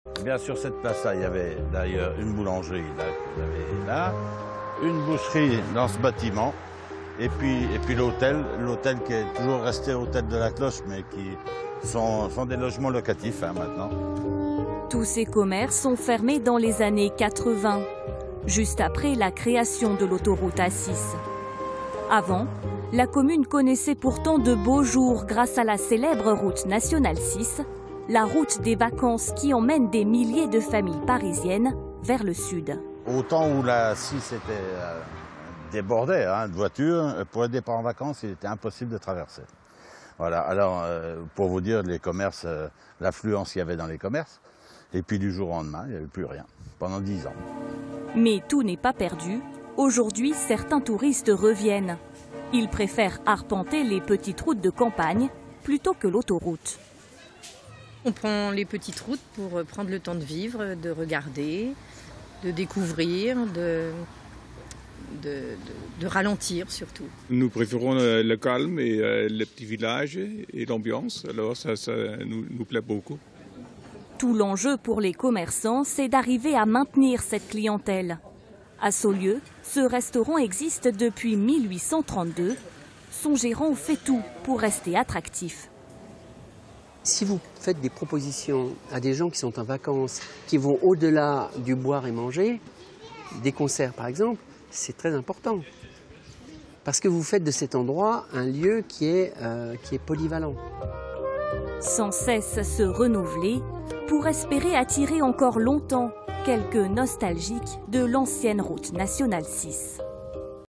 Chassé-croisé : sur les traces de l’ancienne route Nationale 6
En ce mois d’août, les automobilistes sont nombreux à traverser la Bourgogne, notamment pour aller sur la côte méditerranéenne. Nous nous intéressons à l’ancienne route Nationale 6. Elle qui avait connu des beaux jours à l’époque des premiers congés payés. Mais depuis la création de l’autoroute A6 dans les années 1970, certaines communes ont été délaissées par les touristes.

Le reportage de Marie Jolly, Hélène Assekour et Rachel Nectoux avec
Bernard Perreau, maire de La Roche-en-Brenil (SE)
Jean-Marc Tingaud, gérant du Café Parisien